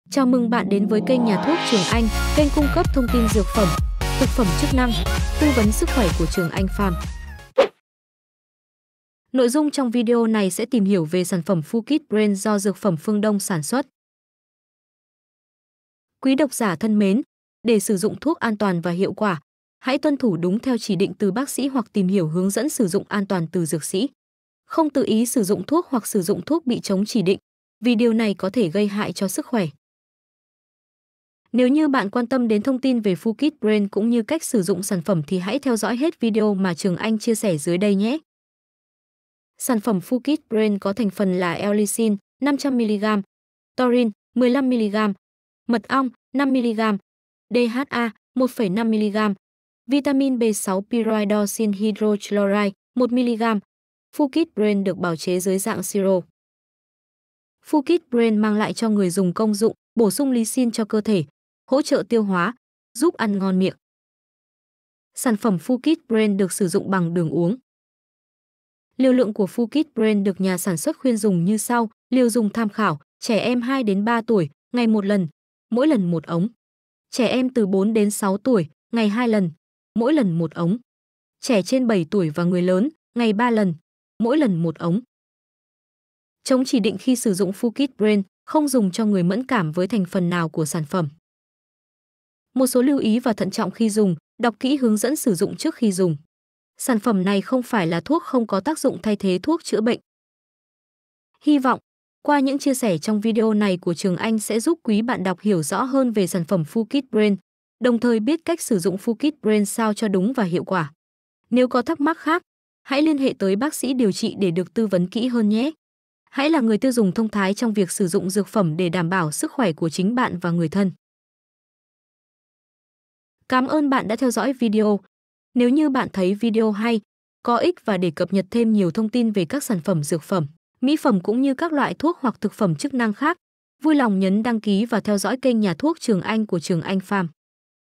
Fudkid Brain Phương Đông - Giúp bổ sung Lysine cho cơ thể
Công dụng
Bổ sung Lysine cho cơ thể, hỗ trợ tiêu hoá, giúp ăn ngon miệng.